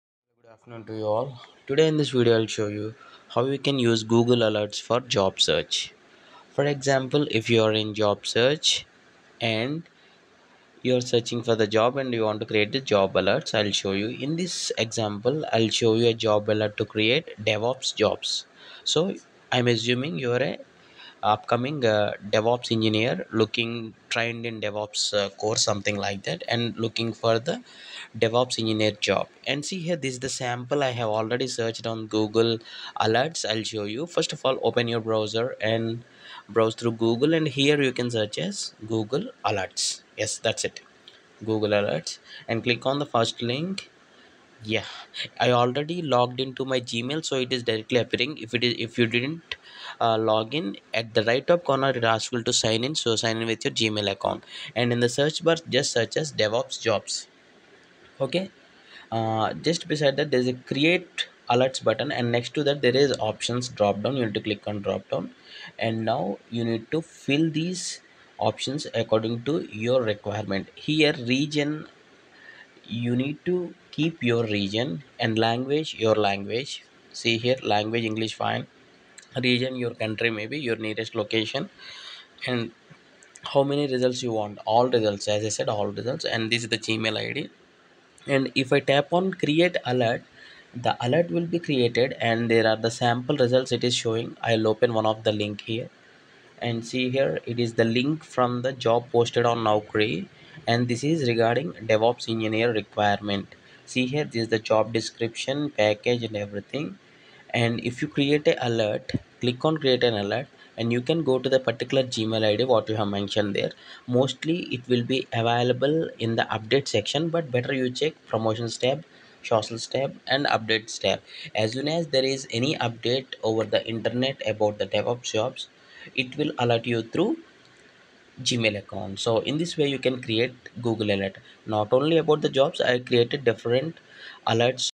how to use google alerts for job search
If you want to set Google alerts for a particular job search as a pressure or experience that candid you can create one Google alert for the job search. in the search bar you need to search for your prefer job and select the options of your location your search results etc and create a job alert.